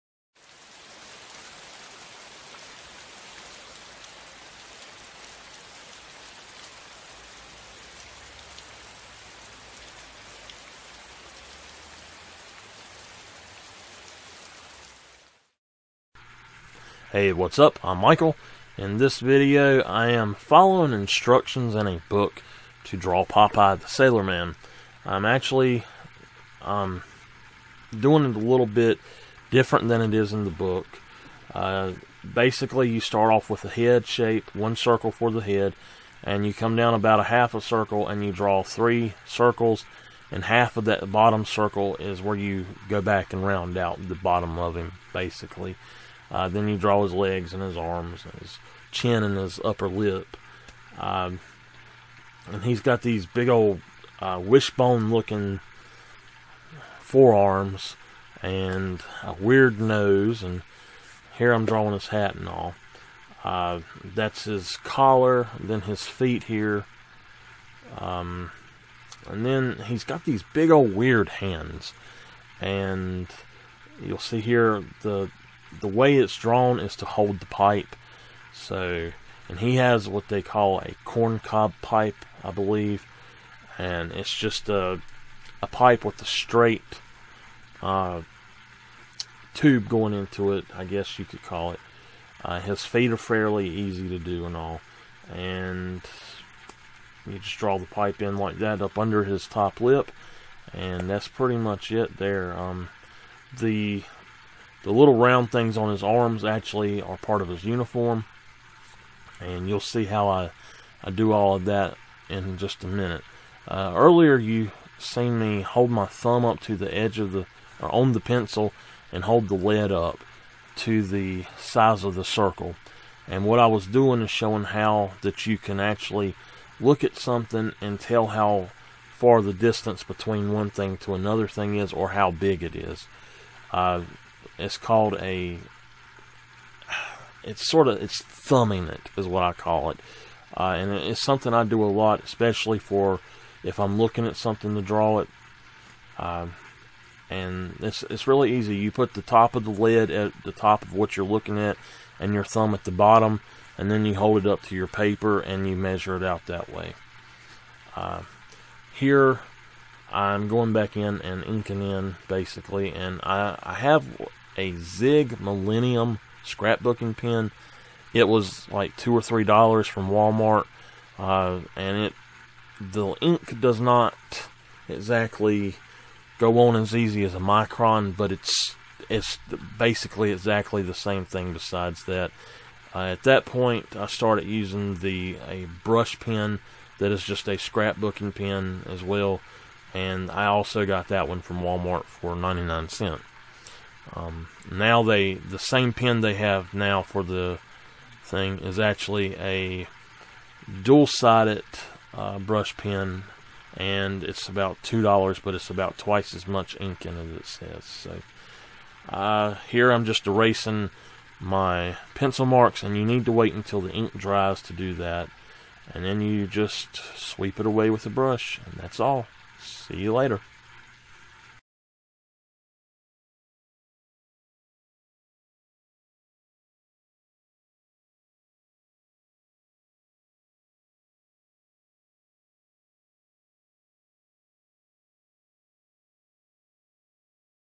Cartooning: Popeye The Sailor Man: Timelapse
This is the first entry into a new series of random cartoon / comic strip type characters. This one is a fan art of Popeye The Sailor Man.   I have an old book that I bought at a book fair when I was in elementary school that tells how to draw a lot of characters like this.  I followed what the book said somewhat, but put a bit of my own touch into it as well.